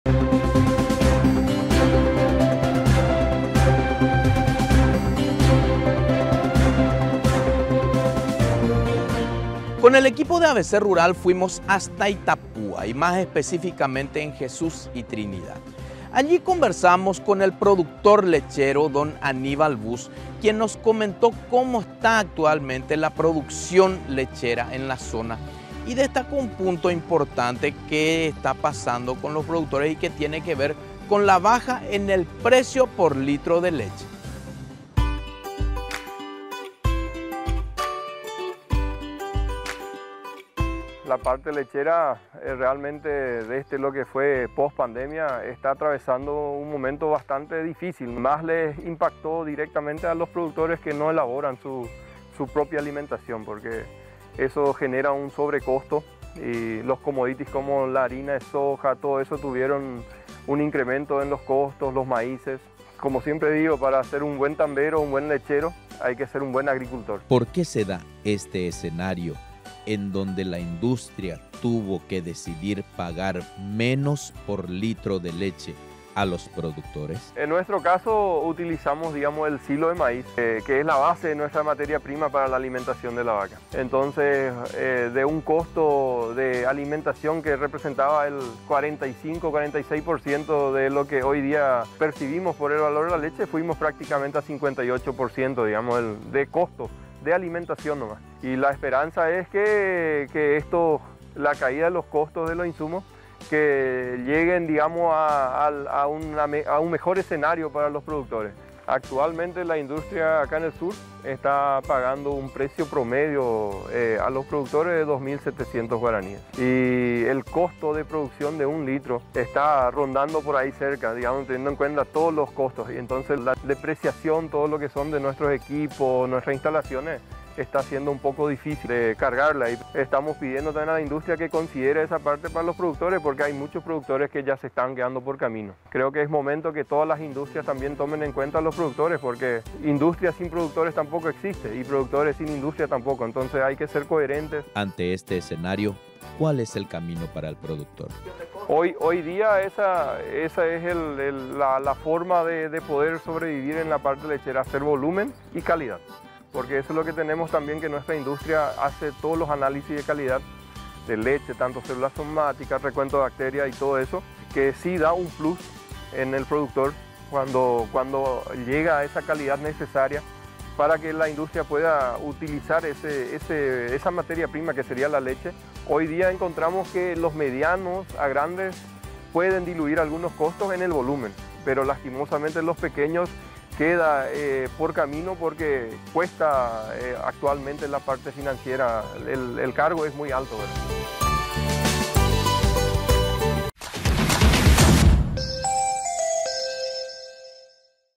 Productores lecheros absorben aumentos de costos de la industria
Aquel que cree que el trabajo de un productor es fácil es solo porque no lo conoce, pues debe lidiar con los precios, el clima, el trabajo dentro y fuera de sus unidades de producción y muchas cosas más. Recientemente, la industrial lechera bajo G. 100 el pago por litro de leche a los productores, y si bien se justifica con el aumento de insumos y otros puntos, en este material el tambero y agricultor Aníbal Buss, destaca que el único camino para el productor es ser cada vez más eficiente y lograr una mayor producción con lo que tiene.